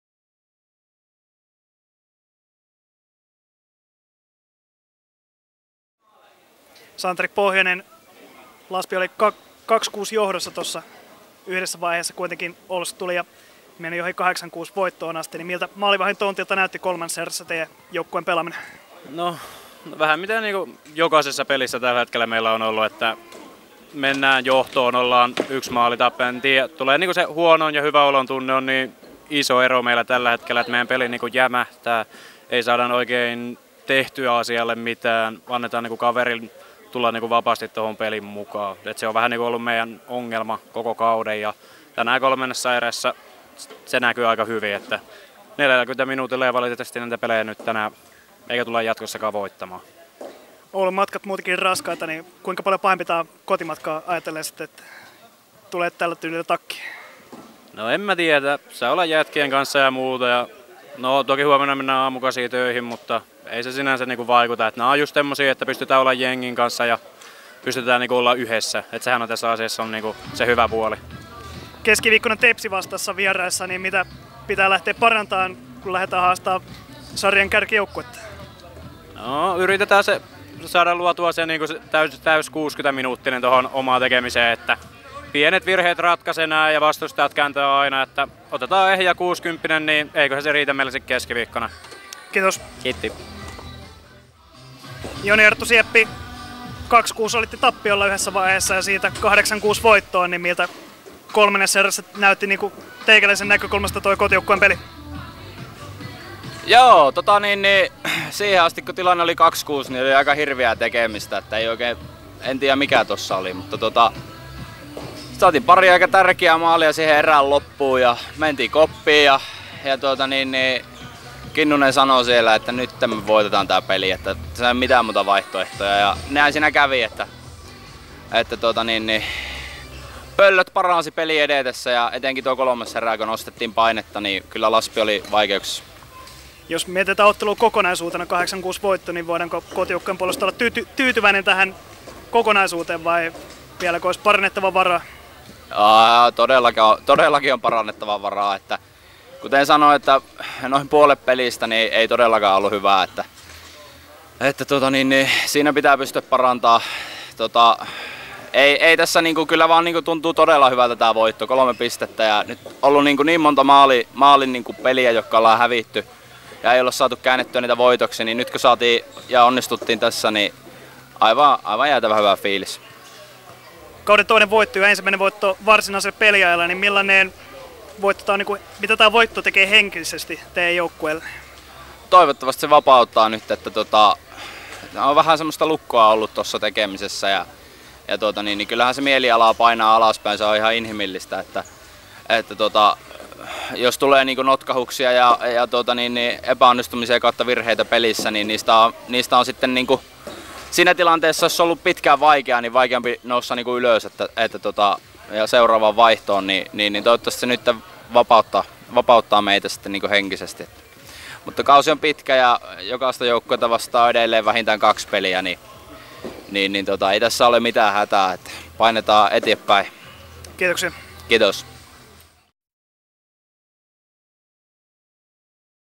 Kommentit pelistä OLS - LASB (8-6) 16.10.2022
OLS:n hoviselostaja/-toimittaja Topias Setälän jutulla käväisivät 15 kertaa torjunut LASB-vahti Santtu Pohjonen, sekä OLS:lle 2 maalia iskenyt Joni-Arttu Sieppi, joka aloitti OLS:n kirin 2. erän lopulla 3-6 maalillaan ja vastasi ottelun voittomaaliksi jääneestä 7-6 osumasta.